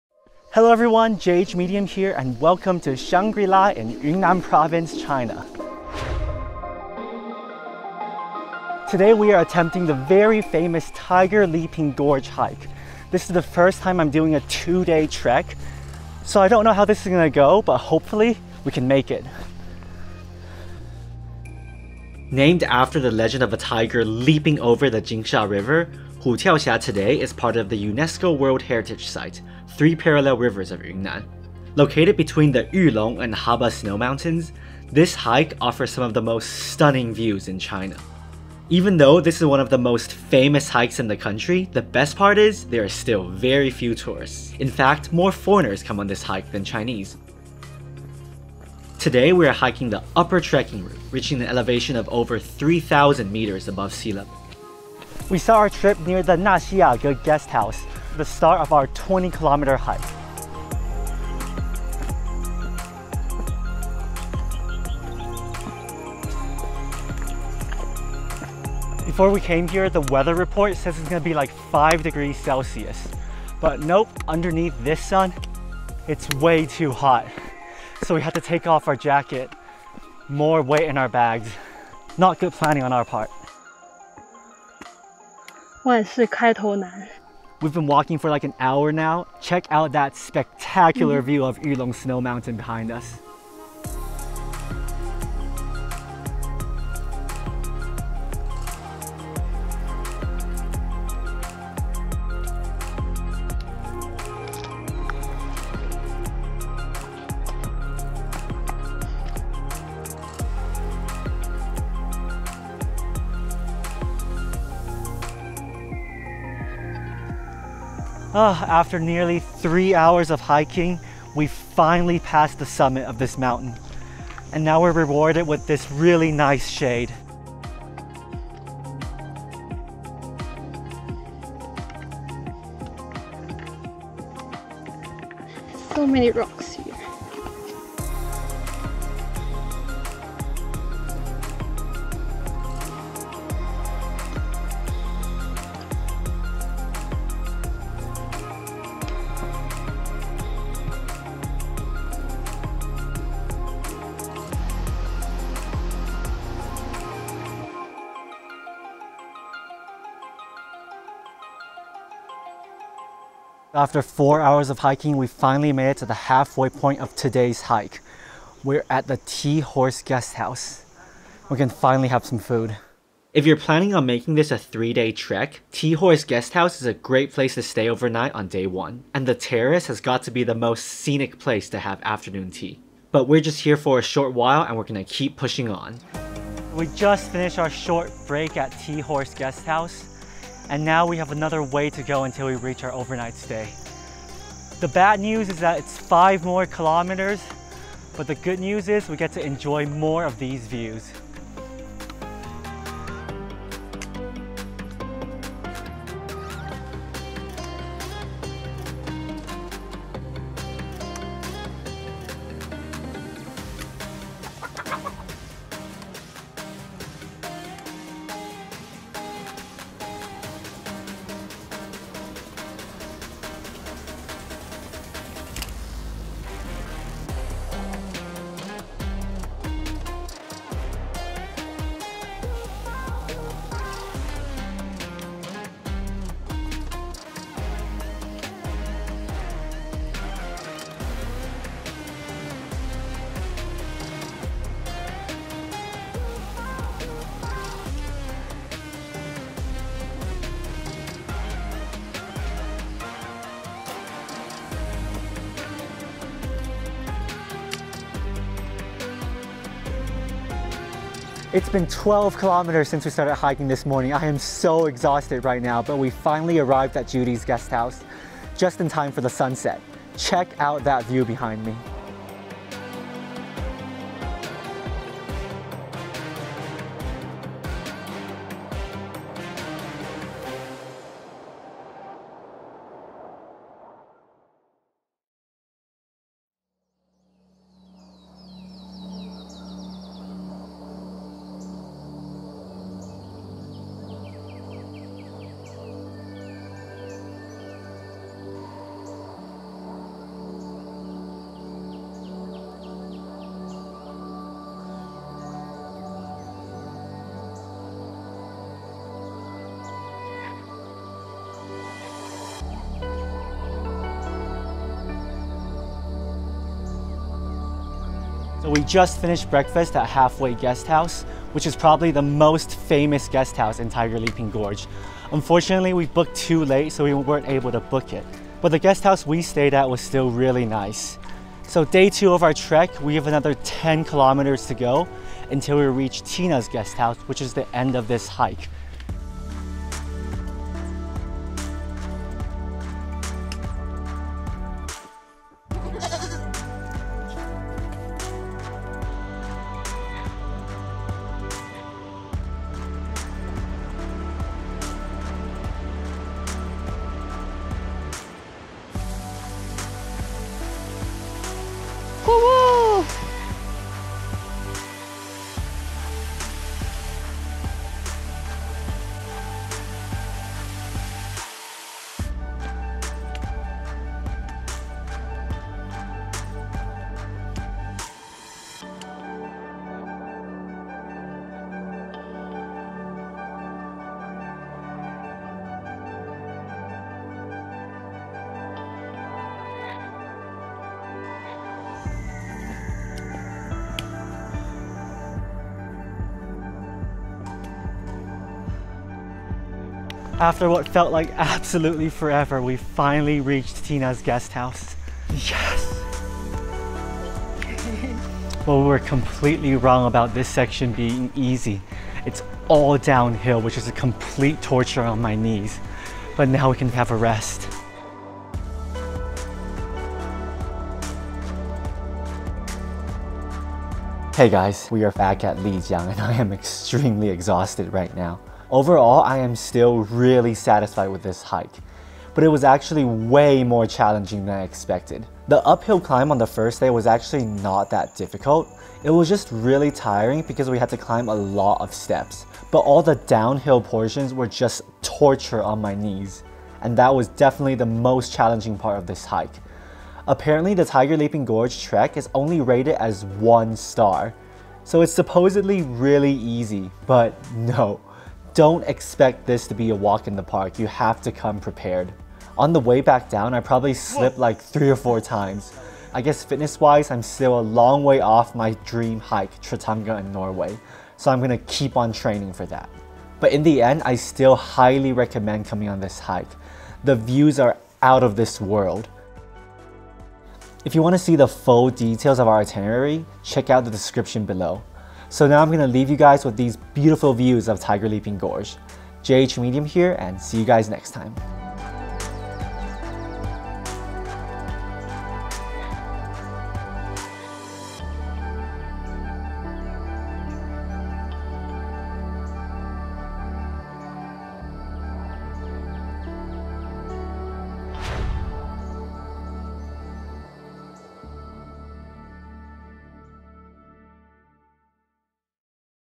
The Best Hike in China | Tiger Leaping Gorge
Named after the legend of a tiger leaping over the Jinsha river, 虎跳峡 Tiger Leaping Gorge is one of the most famous hiking routes in China.  Located between the Yulong and Haba Snow Mountains, this hike offers some of the most stunning views in the country.  The best part is, there are very few tourists.  Join me on this two day hike across Shangri-La County in Yunnan Province.

Hiking shot using the new Insta360 X3.

OUR ITINERARY:
Day 1:
🚌 Lijiang Bus Station 丽江客运站 → Hutiaoxia 虎跳峡镇 (3hrs)
🚙 Cab from Hutiaoxia Village → Naxiyage GH (10mins car or 1hr trek)
🥾 Hike from Naxiyage GH → Tea Horse GH (4hrs)
☕️ Rest and food break at Tea Horse GH (1hr)
🥾 Hike from Tea Horse GH → Judy’s GH (2hrs)
🛌 Dinner and overnight stay at Judy’s GH
Total Hiking Distance:  12km

Day 2:
🍜 Breakfast at Halfway GH
🥾 Hike from Halfway GH → Tina’s GH (4hrs)
🍚 Lunch at Tina’s GH (1hr)
🥾 Middle Tiger Leaping Gorge Roundtrip hike (3hrs)
🚌 Bus from Tina’s GH → Lijiang (3hrs)
Total Hiking Distance:  10km

GEAR:
Main camera:  Insta360 X3
B-Roll camera:  Fujifilm XT4, Sony ZVE10, Insta360 ONE RS
Sound:  DJI Mic